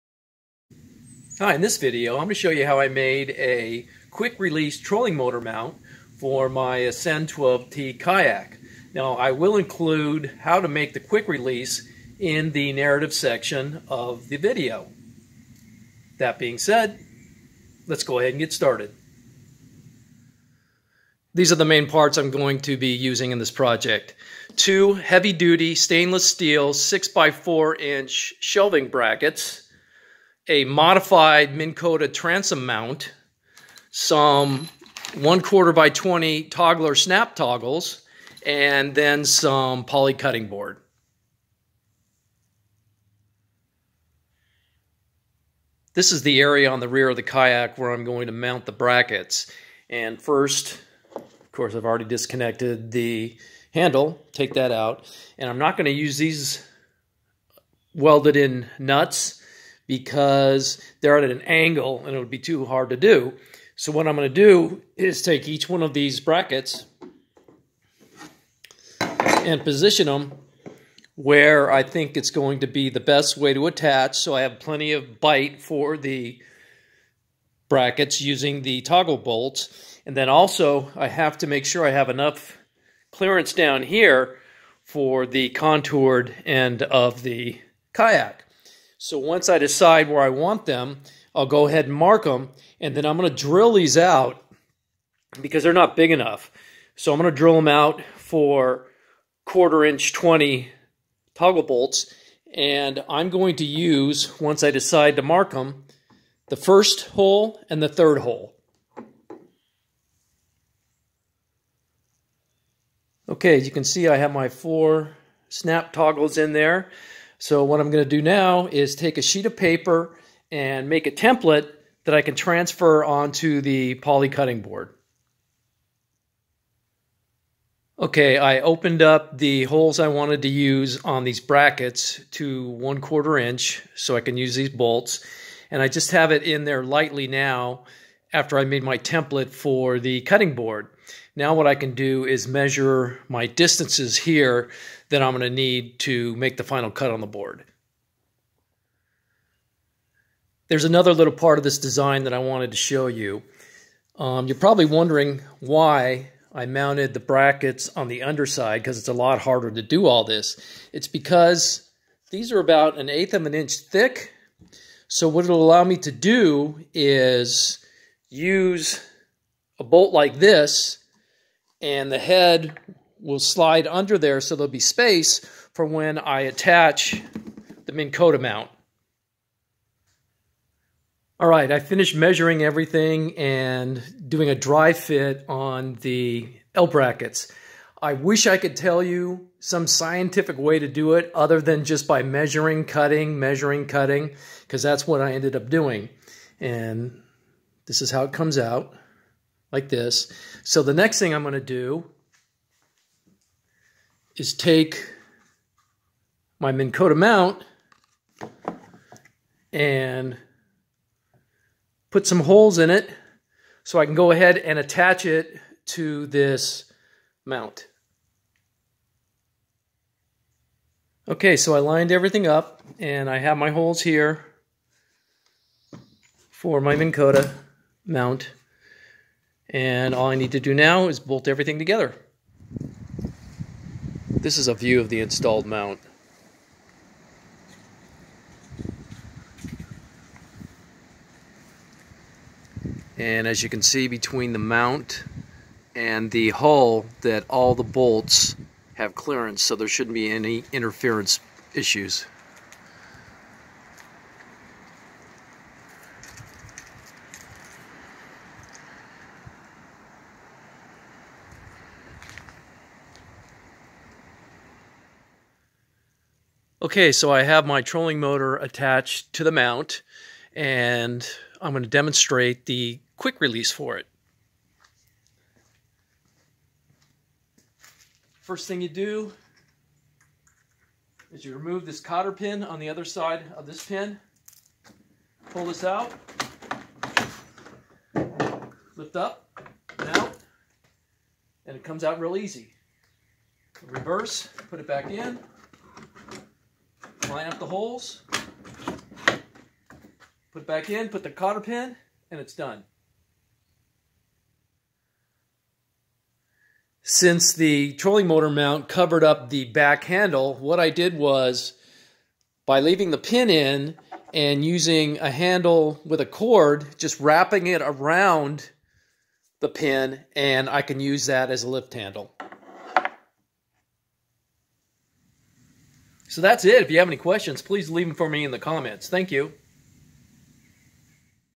DIY Ascend 12T Kayak Trolling Motor Quick Release Mount
This is a video tutorial detailing the design, construction, and installation of a trolling motor mount with a quick release option for an Ascend 12T kayak.

Parts:
* 6" x 4" Heavy Duty Stainless Steel Shelf Bracket.
* 1/4" x 20 Toggler Snap Toggle Bolts (Stainless Steel Bolt Replacements).
* Poly Cutting Board.
* Misc. Stainless Steel Fasteners.

* Quick Release Trolling Motor Mount.